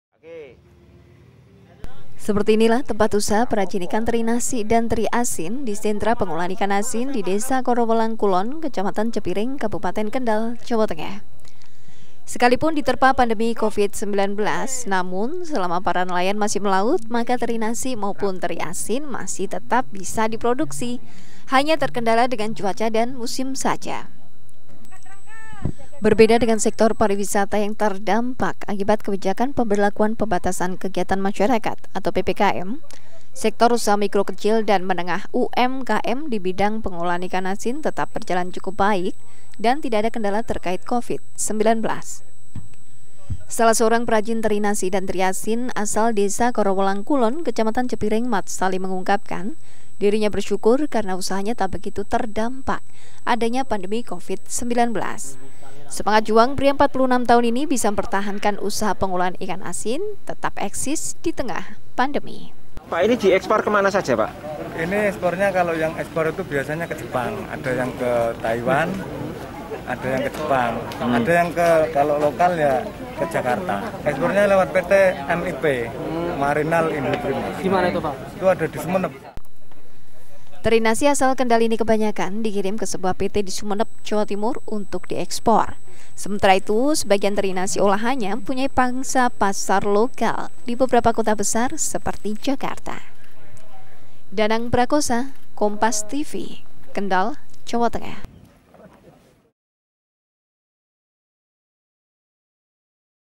Usaha Teri Nasi Kendal Tetap Eksis di Tengah Pandemi Covid 19
KENDAL, KOMPAS.TV - Pandemi Covid-19 yang berlangsung hampir 2 tahun, tak meruntuhkan para  pengusaha teri nasi maupun teri asin  yang berada di Desa Korowelang Kulon, Kecamatan Cepiring, Kabupaten Kendal, Jawa Tengah. Usaha mereka tetap lancar dan diminati masyarakat.

Seperti inilah tempat usaha perajin ikan teri nasi dan teri asin, di sentra pengolahan ikan asin, Desa Korowelang Kulon, Kecamatan Cepiring, Kabupaten Kendal, Jawa Tengah. Sekalipun diterpa pandemi Covid, namun selama para nelayan masih melaut  maka teri nasi maupun teri asin masih tetap bisa diproduksi, hanya terkendala dengan cuaca  dan  musim saja.

Berbeda dengan sektor pariwisata yang berdampak akibat kebijakan Pemberlakuan Pembatasan Kegiatan Masyarakat (PPKM), sektor Usaha Mikro Kecil dan Menengah (UMKM) di bidang pengolahan ikan asin tetap berjalan cukup baik dan  tidak ada  kendala terkait Covid-19.

Salah seorang perajin  teri nasi dan  teri  asin asal Desa Korowelang Kulon, Kecamatan Cepiring, Mad Salim mengungkapkan dirinya bersyukur karena usahanya tak begitu terdampak adanya pandemi Covid-19. Semangat juang pria 46 tahun ini bisa mempertahankan usaha pengolahan ikan asin tetap eksis di tengah pandemi.
"(Pak ini diekspor kemana saja pak?) ini ekspornya kalo yang ekspor biasanya ke Jepang ada yang ke Taiwan, kalo yang lokal ya di Jakarta" ujar Mad Salim, pengusaha ekspor teri nasi dan teri asin.

Teri nasi asal Kendal ini kebanyakan dikirim ke sebuah PT di Sumenep, Jawa Timur untuk diekspor. Sementara itu sebagian teri nasi olahannya mempunyai pangsa pasar lokal di beberapa kota besar, seperti Jakarta.